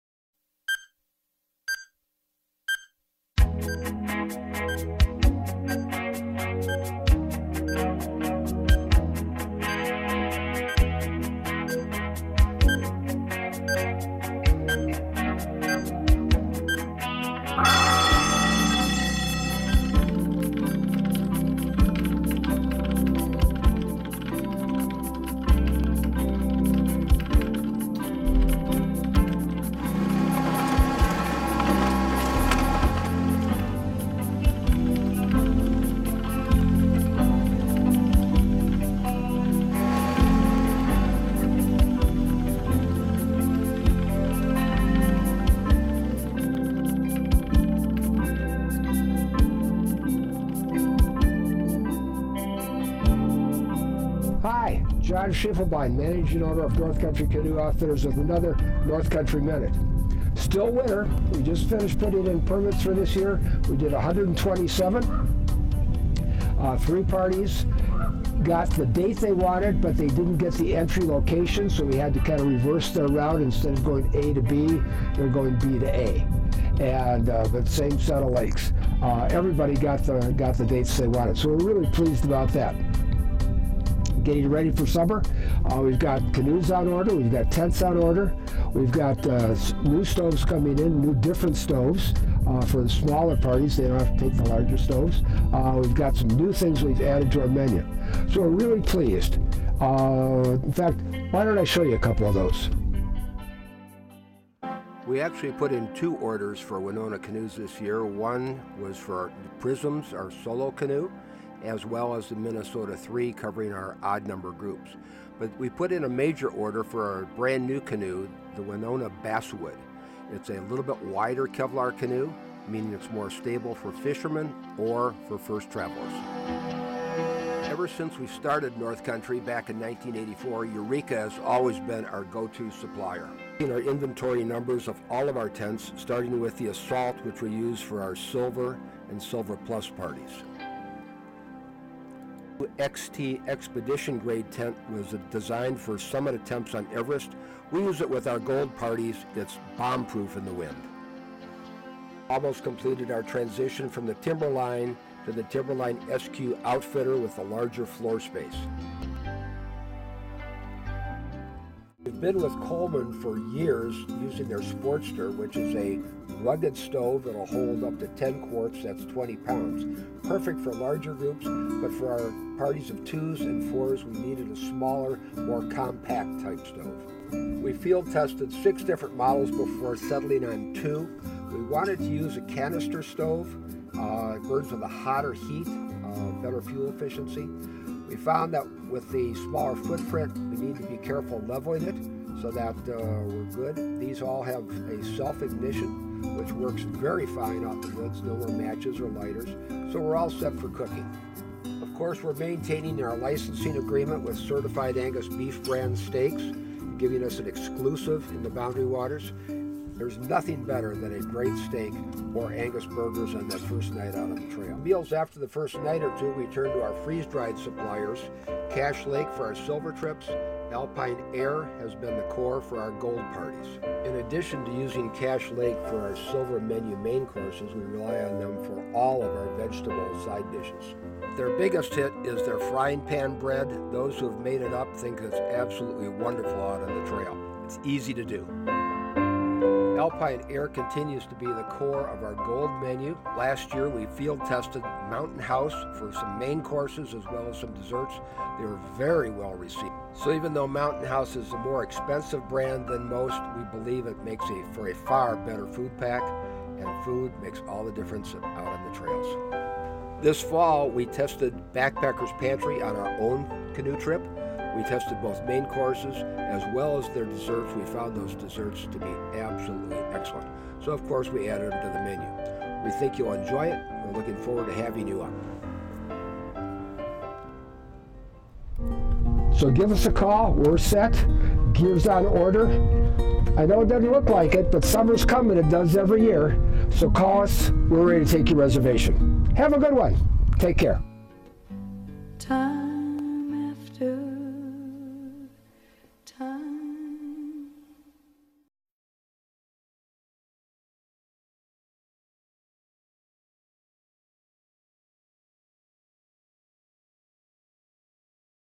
North Country's February 2021 Video Newsletter
Boundary Waters Permits on "Go Live" date.  BWCA canoes, tents, stoves, and food for the 2021 canoe season. BOUNDARYWATERS.com